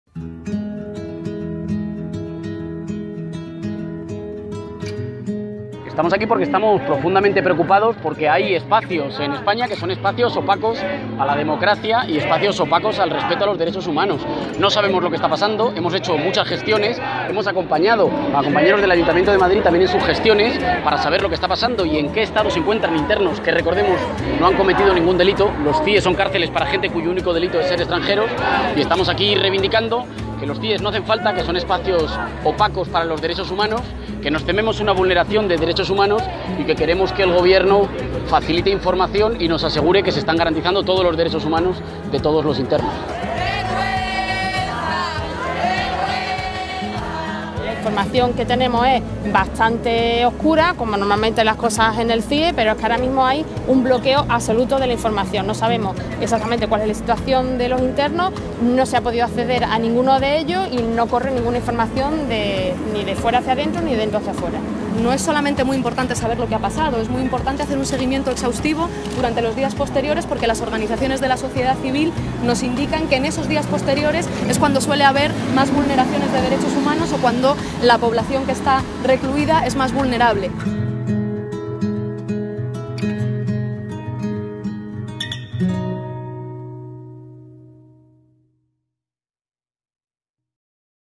Cargos públicos de Podemos asisten a la concentración frente al CIE de Aluche
19 de Octubre de 2016